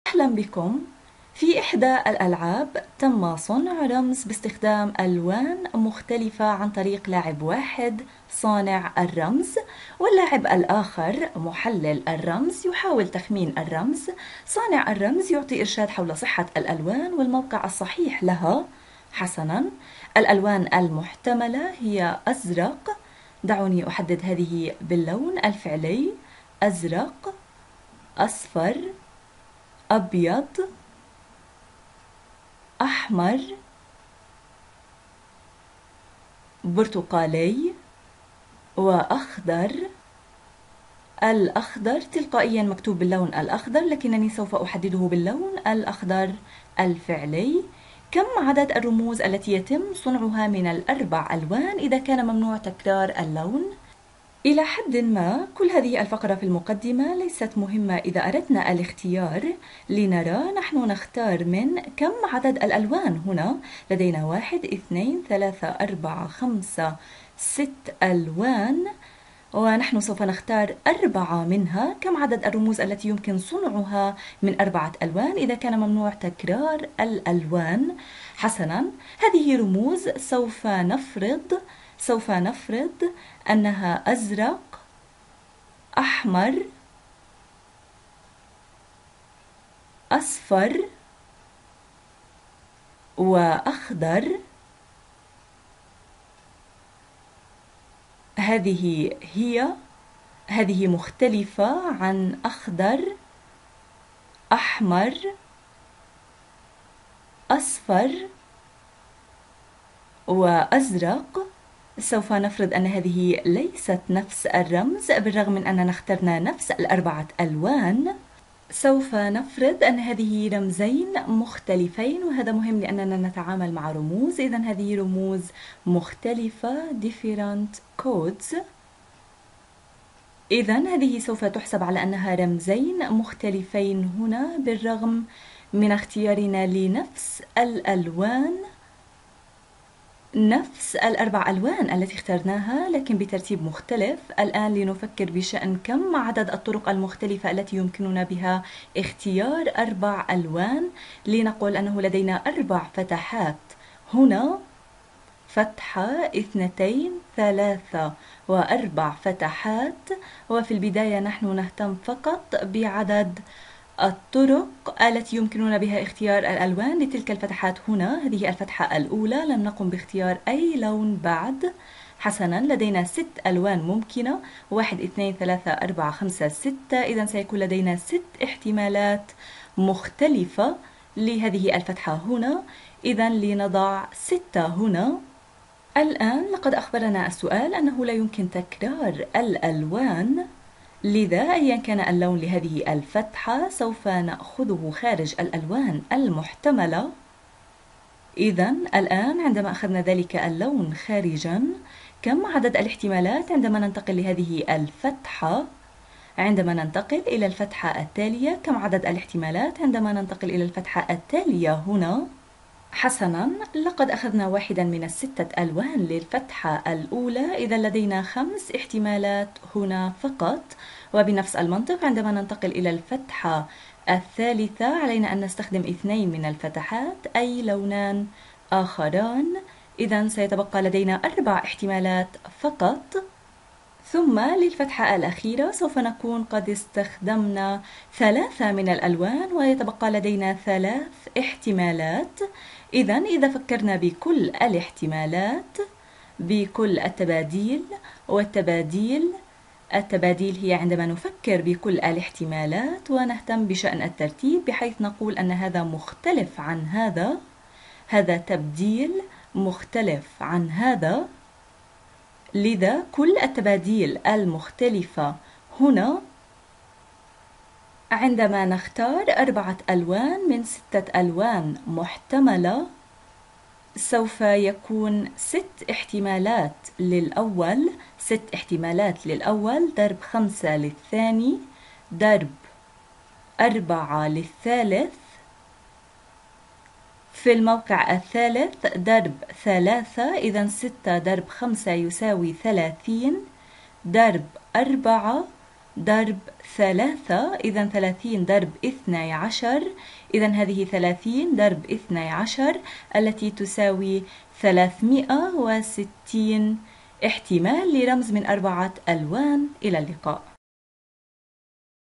التوافيق والتباديل 1
Permutations and Combinations 1
يتناول هذا الفيديو حل مسألة حول الاحتمالات في التباديل والتوافيق
شكر خاص لمؤسسة شركاء في التنمية المستدامة -- فلسطين